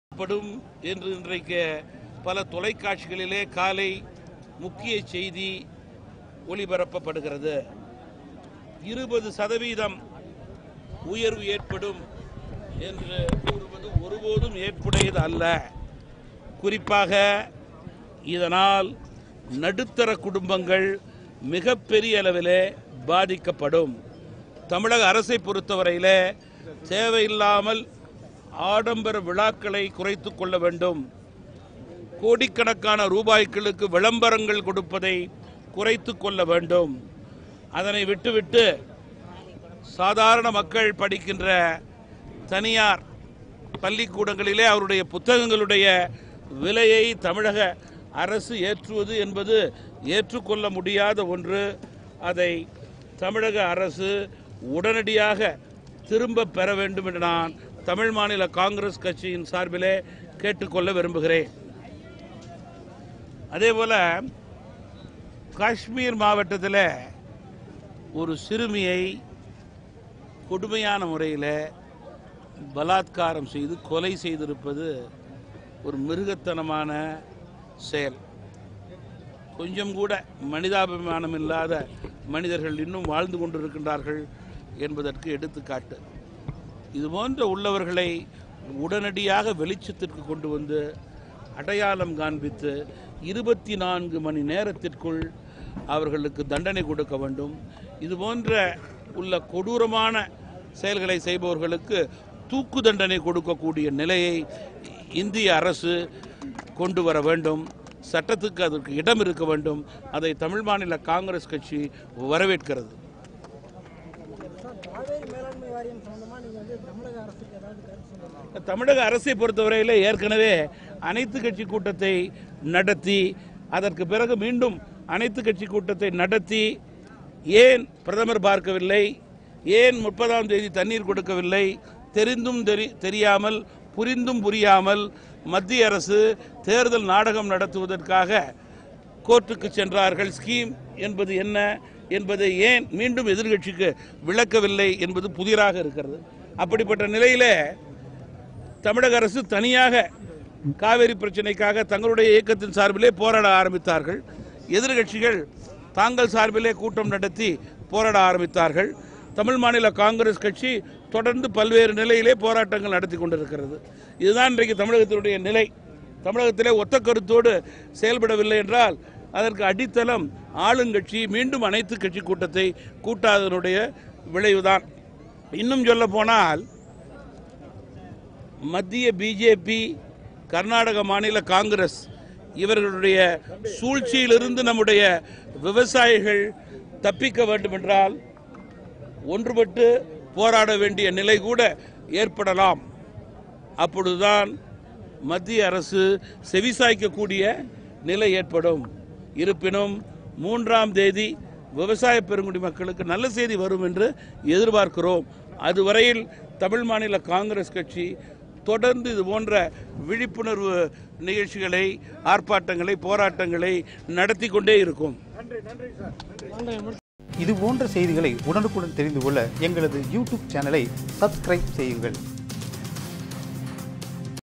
GK vasan angery speech at cauvery issue | தொடர்போராட்டம் நட்த்துவேன்- ஜி.கே. வாசன்
சேலம் மாவட்டம் மேட்டூர் அணை முன்பு தமிழ் மாநில காங்கிரஸ் கட்சி சார்பில் கண்டன ஆர்ப்பாட்டம் நடைபெற்றது. ஆர்ப்பாட்டத்தில் தமாகா தலைவர் ஜி.கே.வாசன் கலந்து கொண்டு கண்டன உரையாற்றினார். பின்னர், செய்தியாளர்களிடம் பேசிய ஜி. கே., வாசன், தமிழக மக்கள் மற்றும் விவசாயிகளின் நலனை கருத்தில் கொண்டு மத்திய அரசு காவிரி மேலாண்மை வாரியத்தை விரைந்து அமைக்க வேண்டும். இதற்கு மாநில அரசு உரிய அழுத்தம் தர வேண்டும் என தெரிவித்தார். மேலும் இந்தியாவில் சமீப காலமாக இளம் குழந்தைகள் பாலியல் வன்கொடுமைகளுக்கு ஆளாக்கப்படுகின்றனர். இத்தகைய கொடுஞ்செயலில் ஈடுபடுபவர்களை தூக்கில் போட சட்டம் கொண்டுவர வேண்டும் என தெரிவித்தார்.